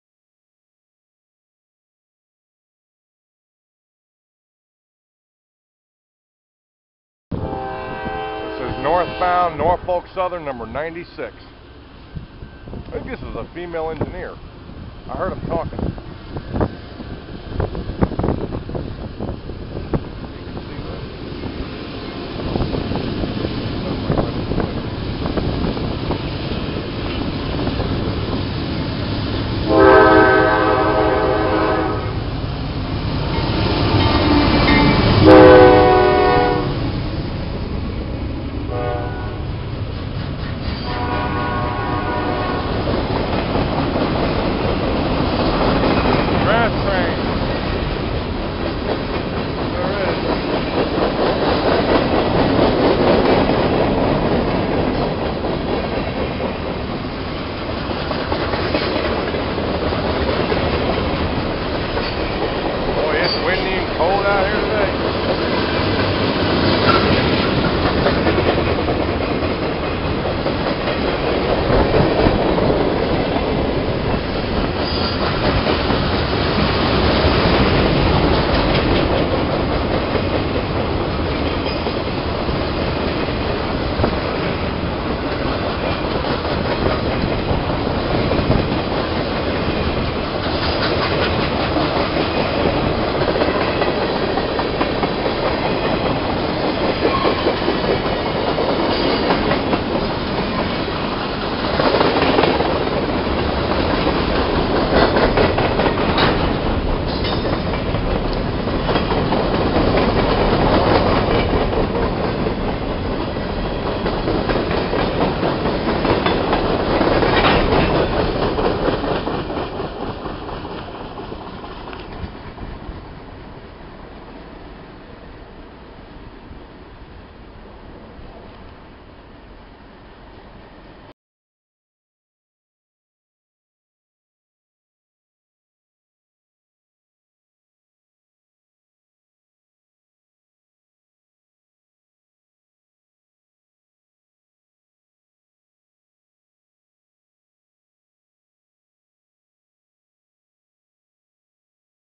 Old 96 Trash.wmv
This is a local NS that hauls what i am thinking is trash scrap metal. I see this smae local all the time. It seemed a little strange though, using a couple of engines to haul this.